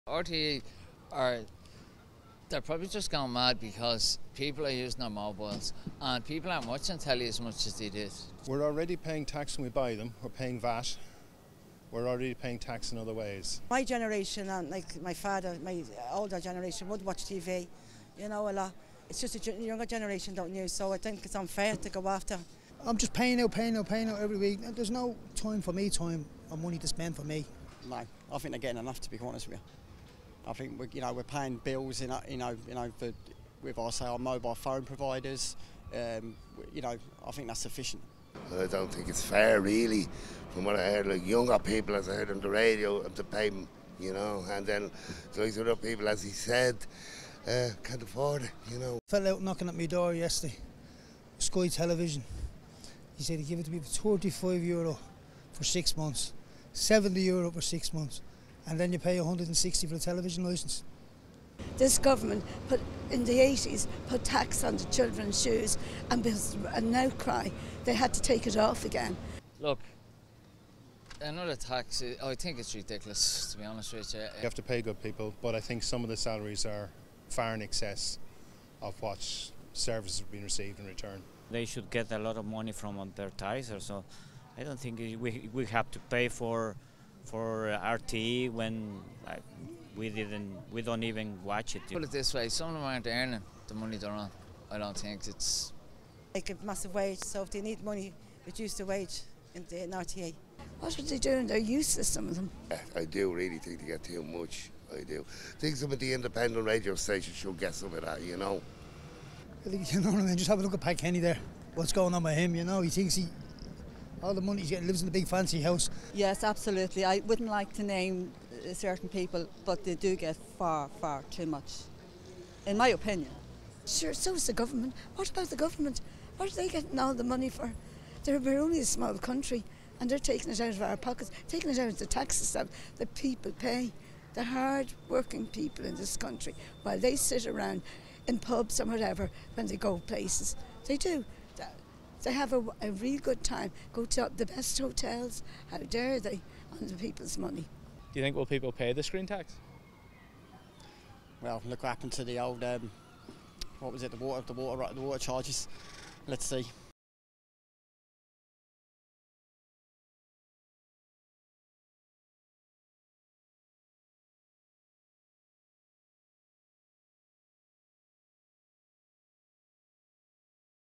The Screen Tax - "How Dare They?!"
People are not too pleased about the new Screen Tax...will the Government face another boycott?